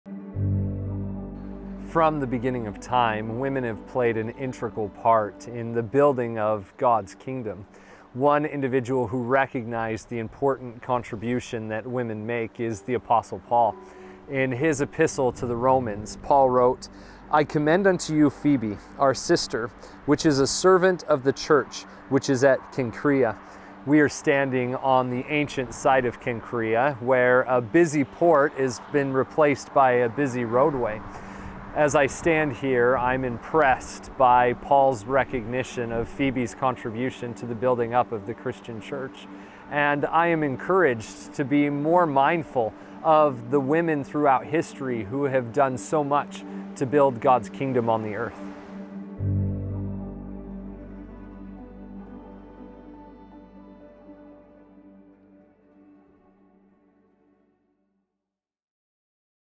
Cenchrea
From the ancient site of Cenchrea, Josh Matson reflects on womens’ efforts to build the kingdom of God.